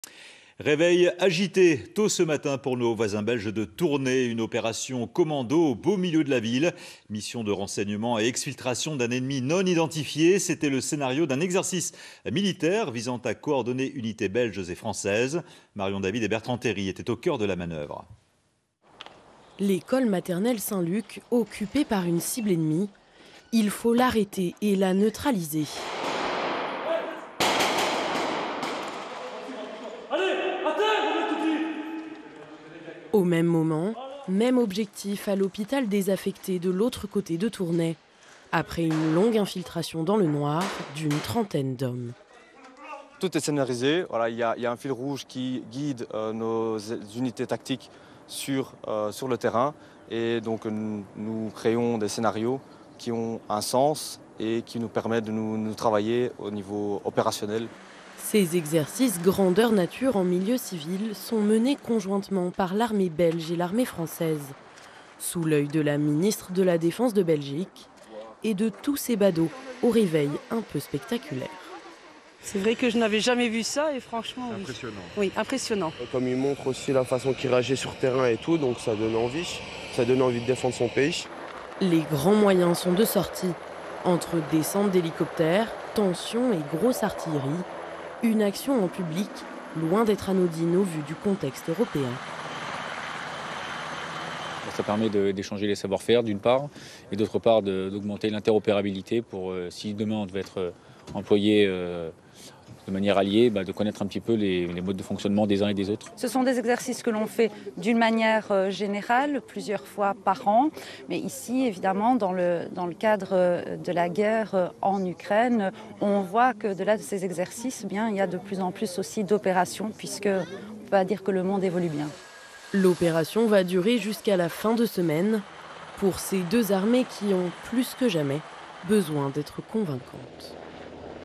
Sécurité : L'armée belge en opération commando à Tournai
Réveil agité tôt ce matin pour nos voisins belges de Tournai. Une opération commando au beau milieu de la ville. Mission de renseignement et exfiltration d'un ennemi non identifié. C'était le scénario d'un exercice militaire visant à coordonner unités belges et françaises.
► Marion David et Bertrand Théry étaient au coeur de la manoeuvre. Montage : Bruno Weill